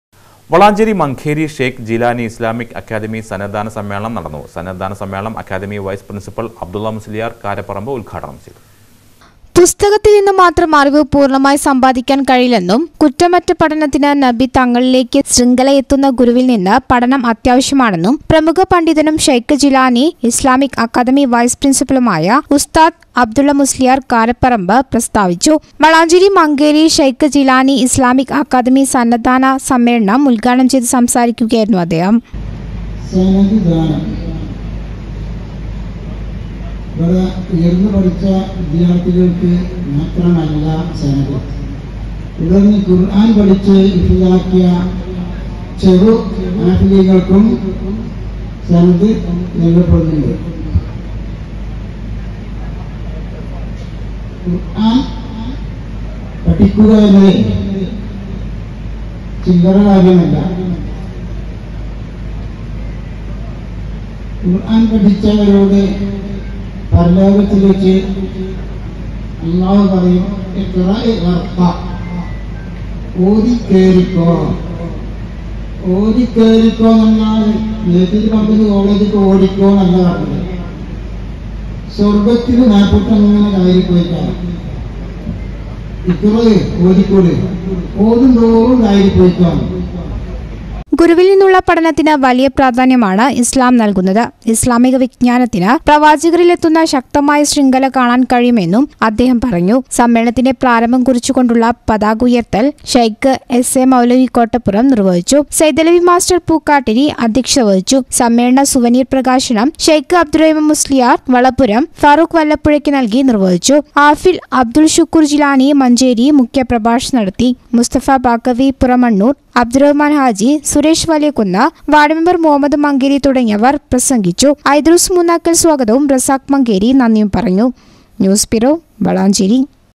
വളാഞ്ചേരി മങ്കേരി ശൈഖ് ജീലാനി ഇസ്ലാമിക് അക്കാദമി സനദ് ദാന സമ്മേളനം നടന്നു
വളാഞ്ചേരി മങ്കേരി ശൈഖ് ജീലാനി ഇസ്ലാമിക് അക്കാദമി സനദ് ദാന സമ്മേളനം നടന്നു.സനദ് ദാനസമ്മേളനം അക്കാദമി വൈസ് പ്രിന്‍സിപ്പല്‍ അബ്ദുല്ല മുസ്ലിയാര്‍ കാരപറമ്പ് ഉദ്ഘാടനം ചെയ്തു.